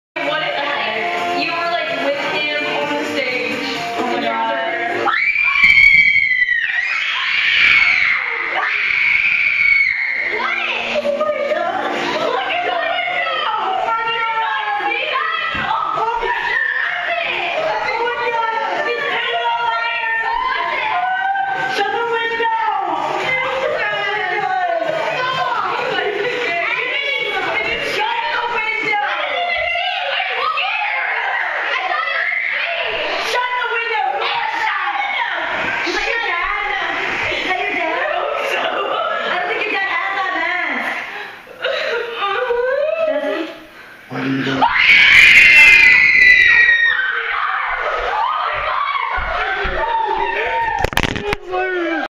Scared by Vendetta Mask
Kids scared by V for Vendetta Mask in window then in kitchen